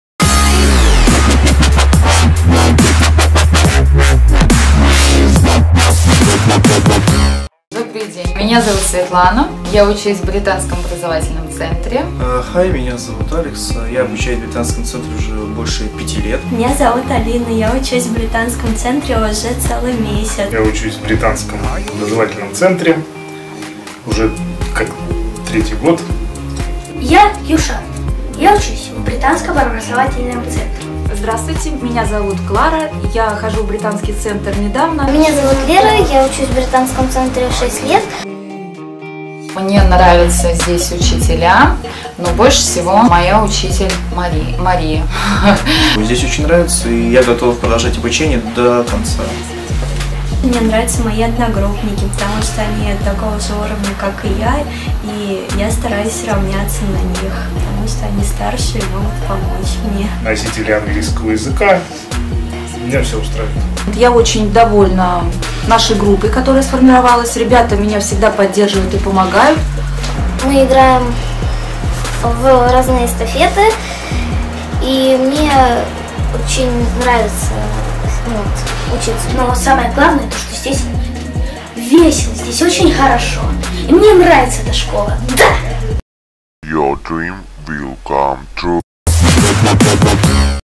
Английский в Железнодорожном и Балашихе. Отзывы студентов о Британском образовательном центре. 0+
Отзывы студентов British Educational Centre - BEC (Британского образовательного центра) о преподавателях и процессе обучения в BEC.
Английский с преподавателями-носителями языка в Балашихе и Железнодорожном.